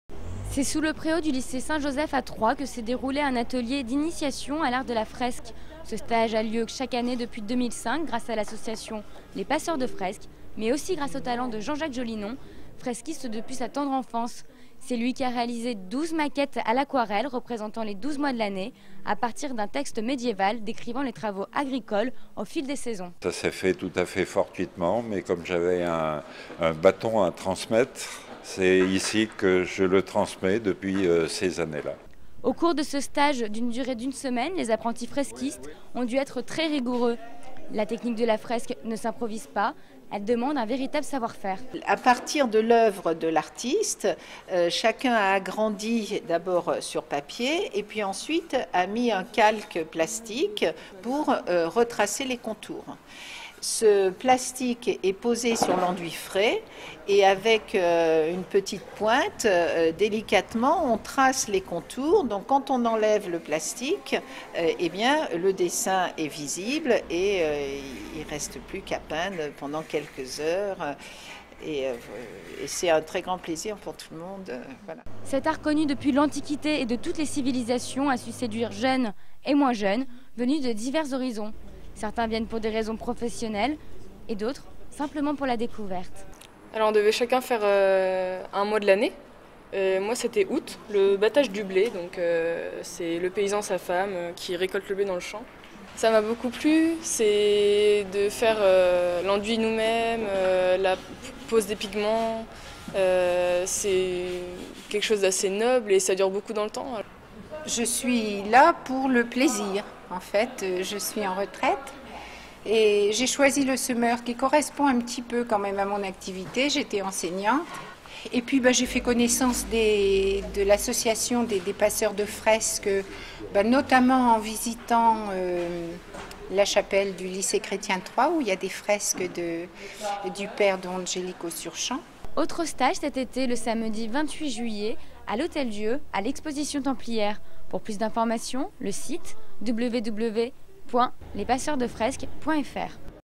Initiation à l'art de la fresque (Troyes)
Sites webs/blogs, copiez cette vidéo avec le bouton "Intégrer" !
 Un atelier d'initiation à la fresque était organisé sur le préau du lycée Saint-Joseph par l'association "Les Passeurs de Fresques"